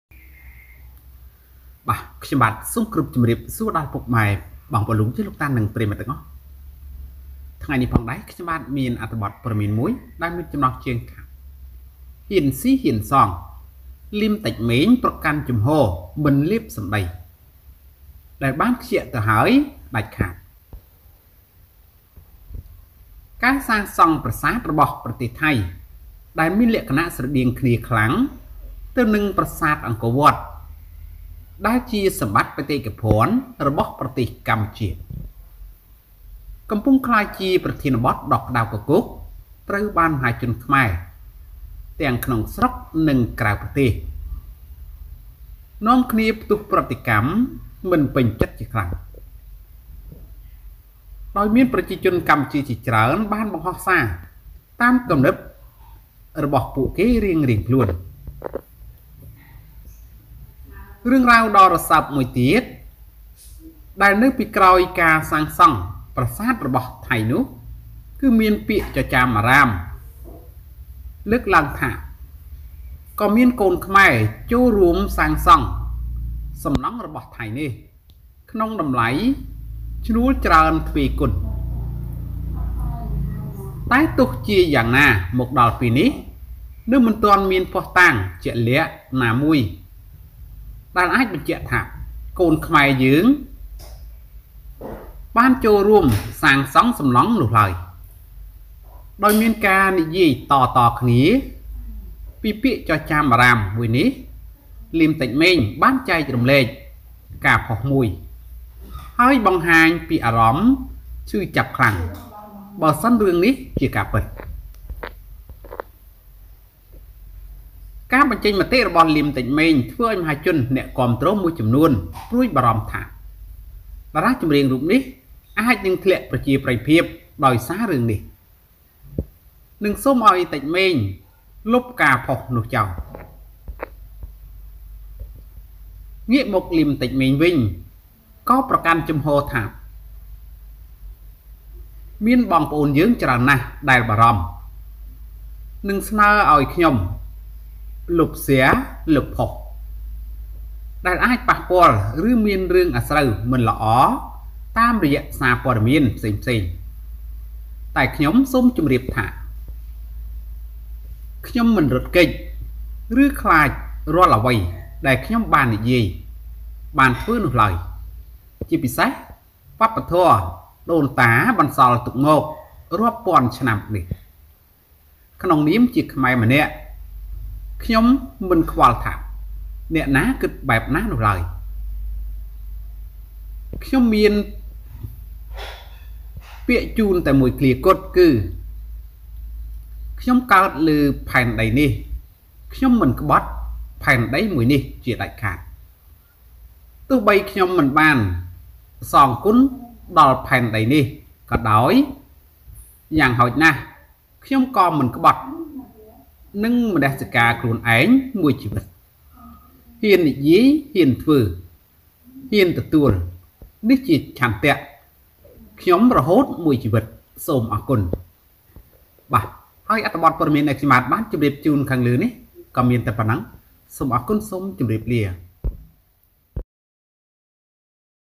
ហ៊ានស៊ីហ៊ានសង! លីម តិចម៉េង ប្រកាន់ជំហរមិនលេបសម្ដី ដែលបានខ្ជាកទៅហើយដាច់ខាត
Dare to eat, dare to pay!  Lim Tech Meng takes a stand not to swallow the words that have been lost